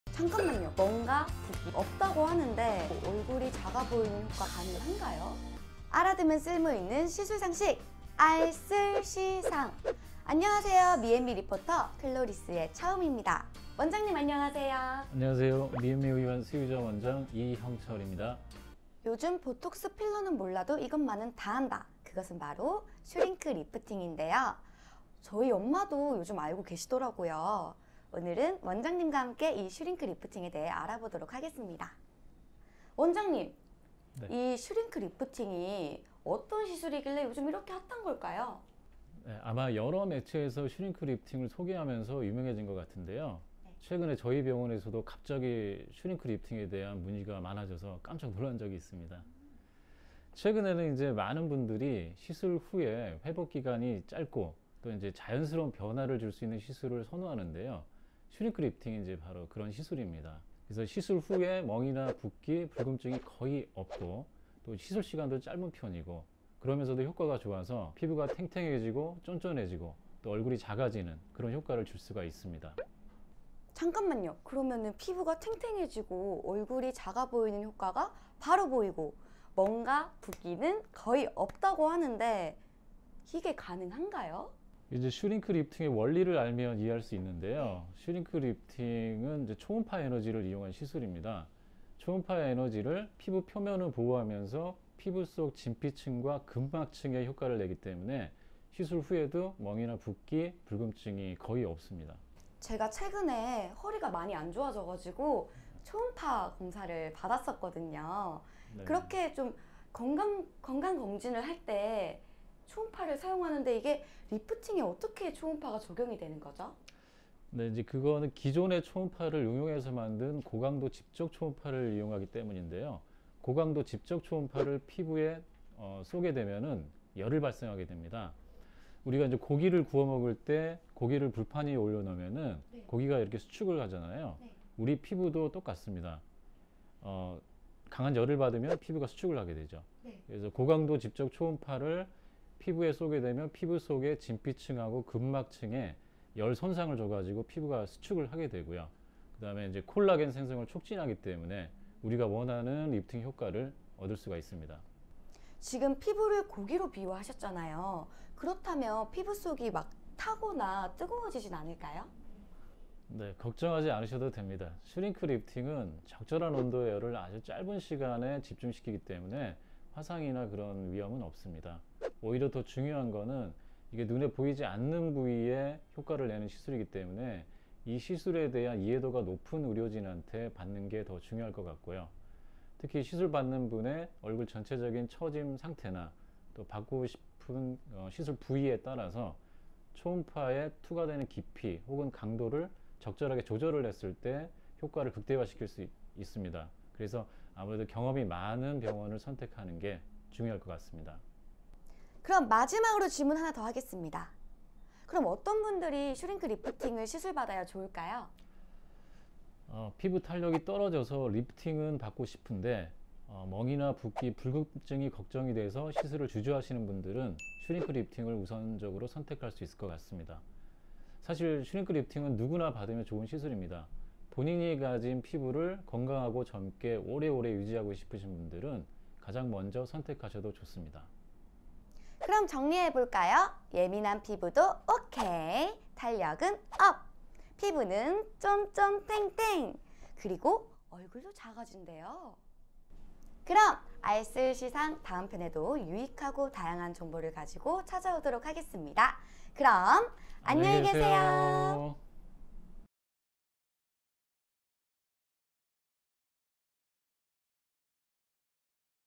[JPN/CHN Sub] 슈링크리프팅 시술상식 알쓸시상! Shulink lifting
알아두면 쓸모있는 시술상식 알쓸시상!
요즘에 많은 분들이 궁금해하시는
슈링크리프팅(Shulink lifting)에 대해 알아보도록 하겠습니다!

미앤미 수유점 이형철 대표원장님과 함께합니다!